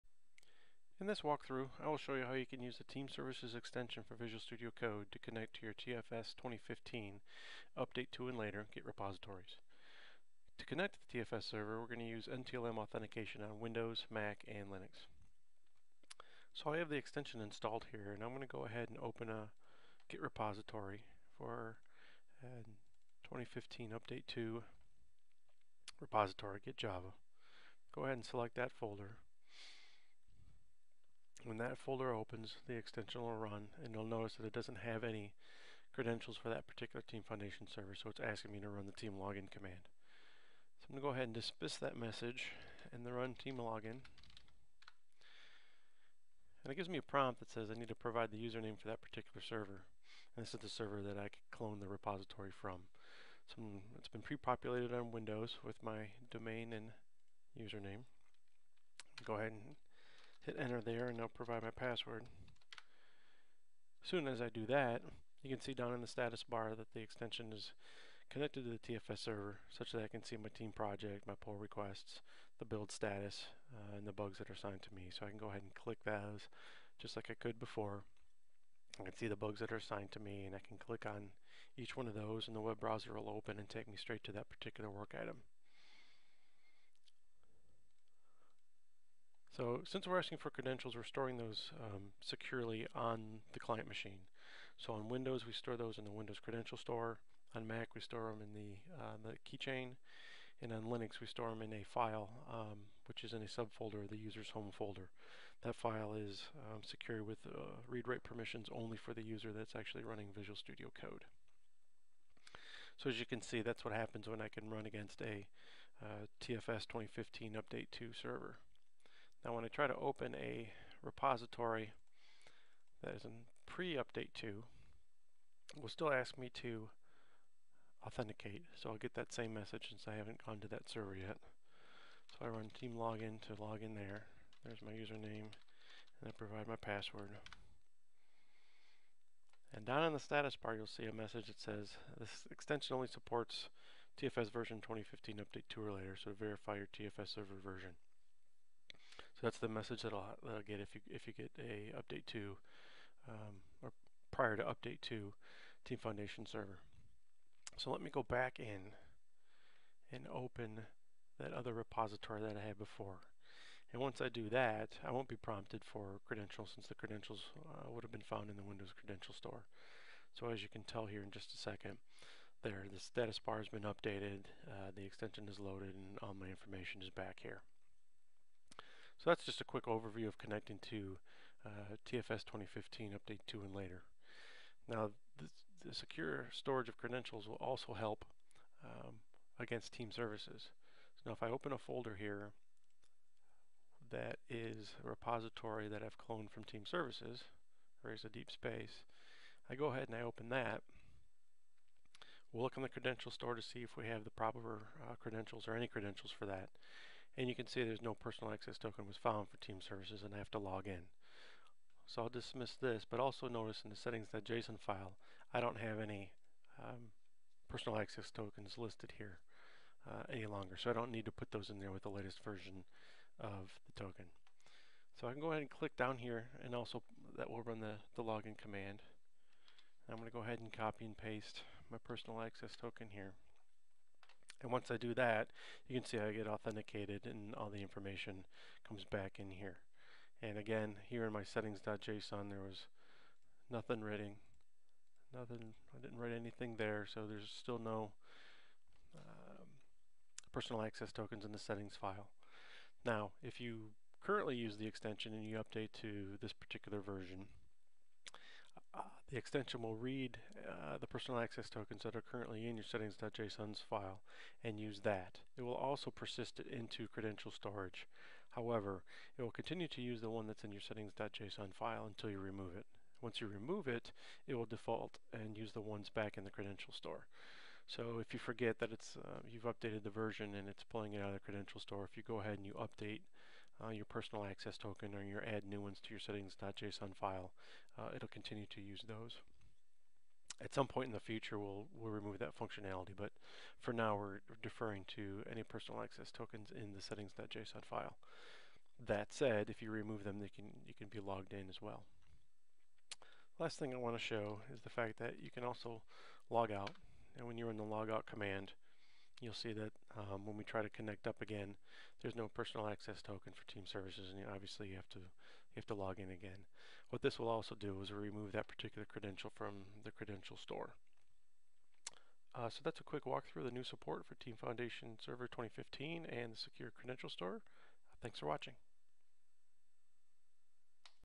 Walkthrough of TFS 2015 support in Team Services extension for Visual Studio Code
This video shows a walkthrough of the Team Foundation Server 2015 Update 2 (and later) support included in the Team Services extension for Visual Studio Code.